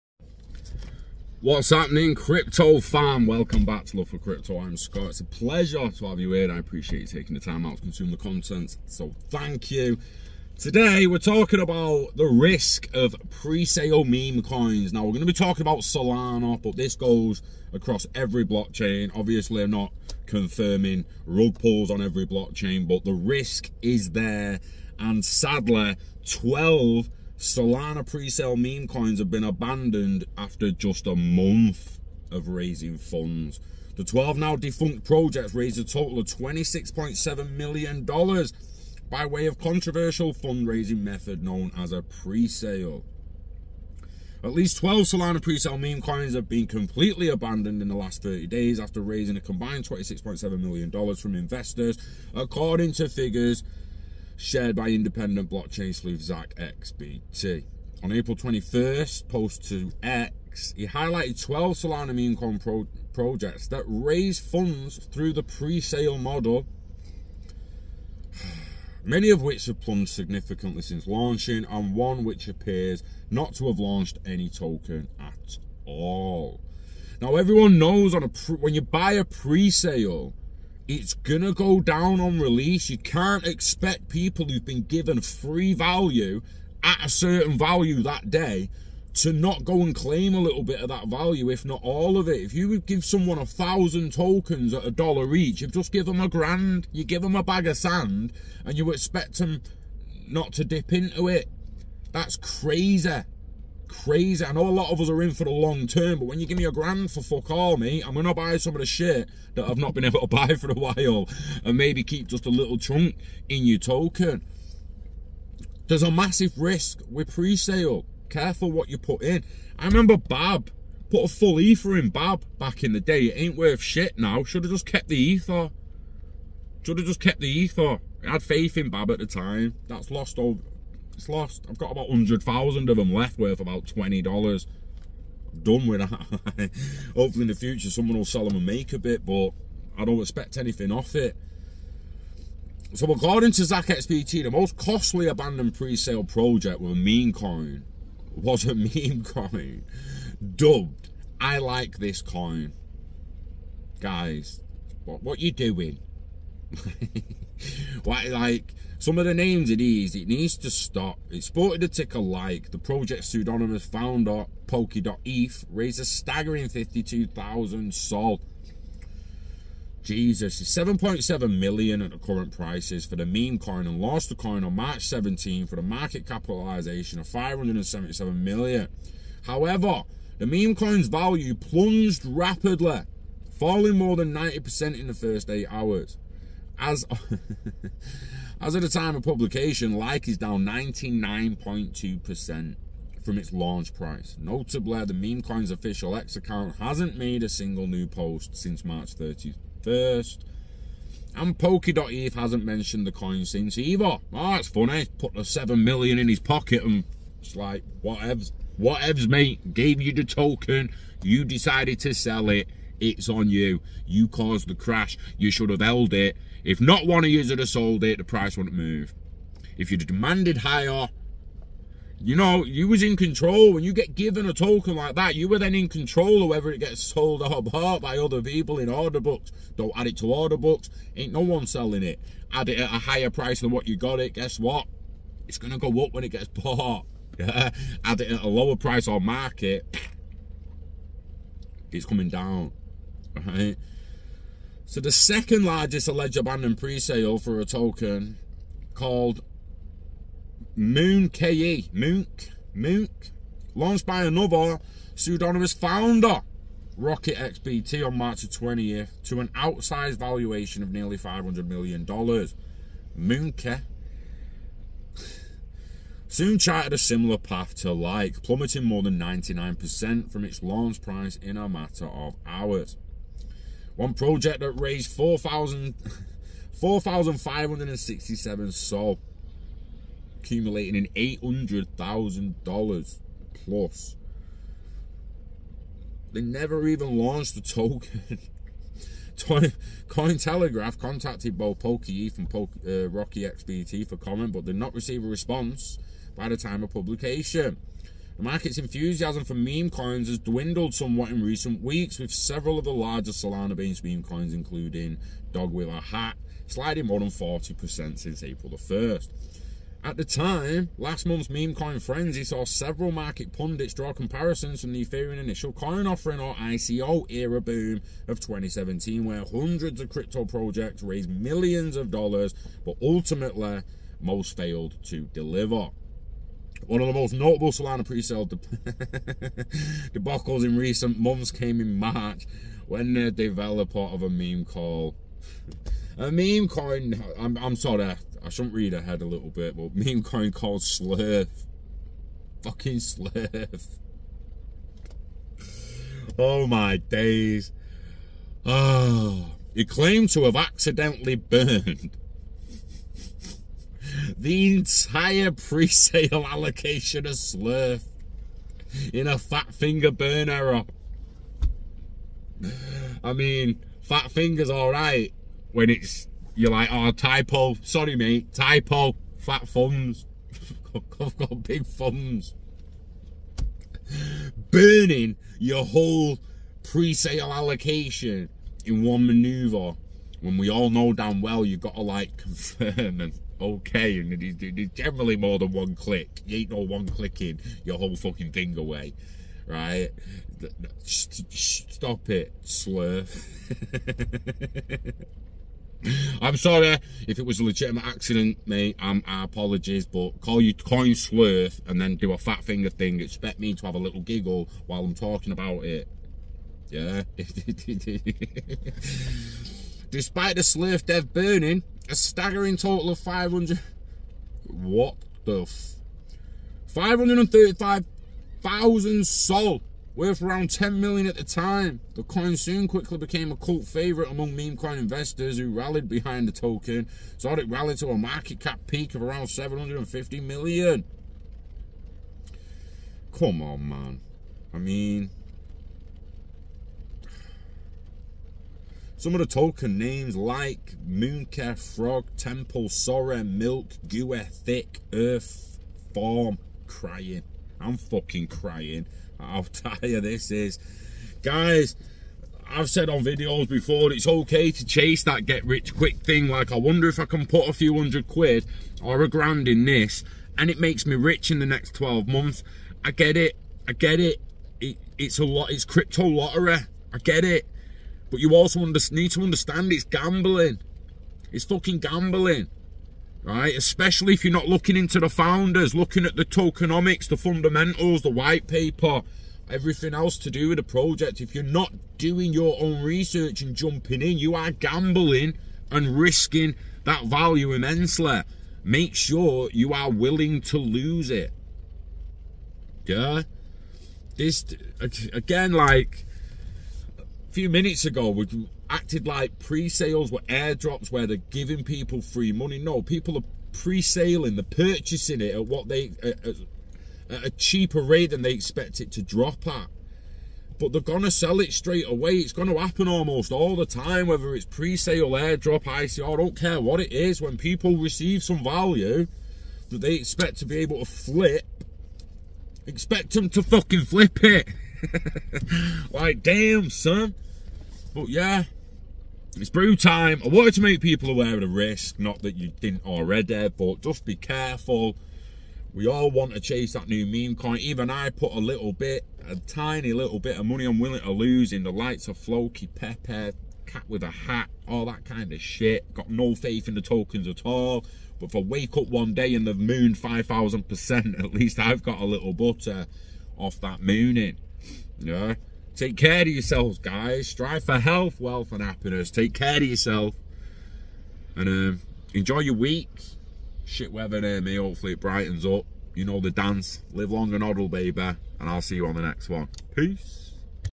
Memecoin Scam Risks 12 Abandoned Sol Projects
At least 12 Solana presale memecoins have been “completely abandoned” in the last 30 days after raising a combined $26.7 million from investors, according to figures shared by independent blockchain sleuth ZachXBT.

In an April 21 post to X, ZachXBT highlighted 12 Solana memecoin projects that raised funds through the controversial presale method, many of which have plunged significantly since launching and one which appears not to have launched any token at all.